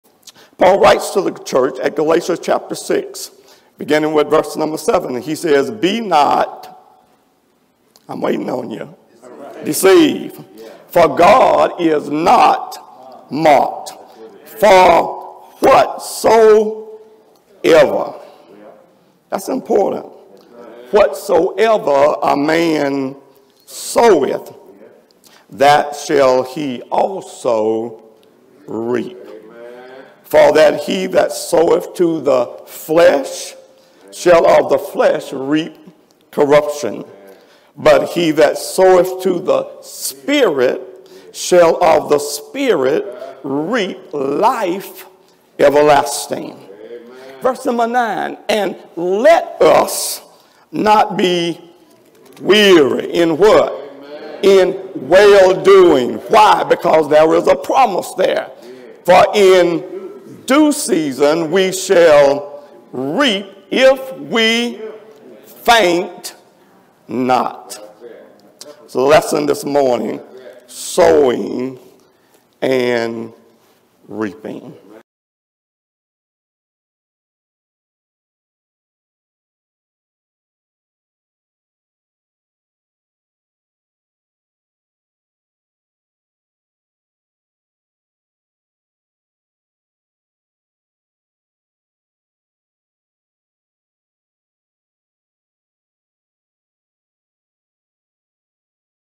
"Season of Sowing" September Revival - "Sowing and Reaping"
Our September Revival series continued this past Sunday with Bro. Kaffee Andrews (minister of the Highway 69 Church of Christ).
Be sure to stream or download the full sermon (audio or video) from our
Don't miss next week's dynamic speaker in "The Season Of Sowing" Fall Revival.